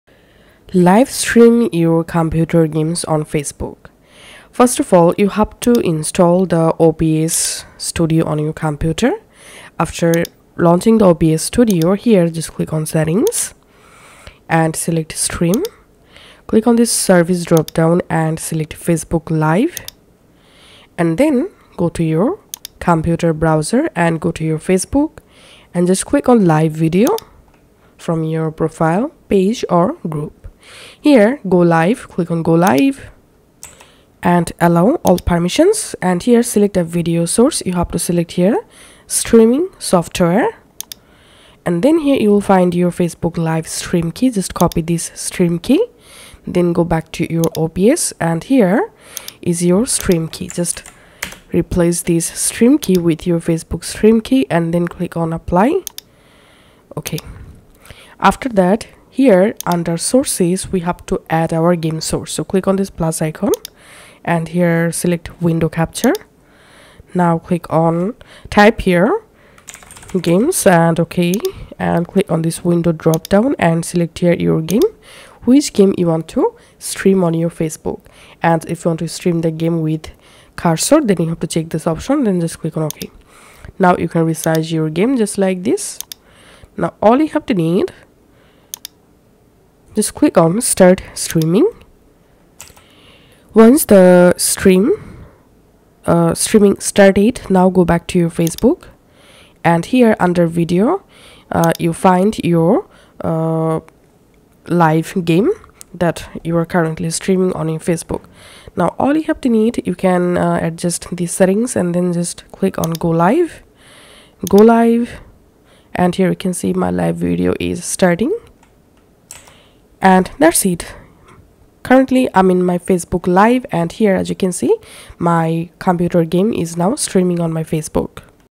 How to Live Stream PC Games on Facebook 2026
Find out how to set up and launch a Facebook Live stream for your gaming video creator Page through Facebook Gaming by using the Creator portal.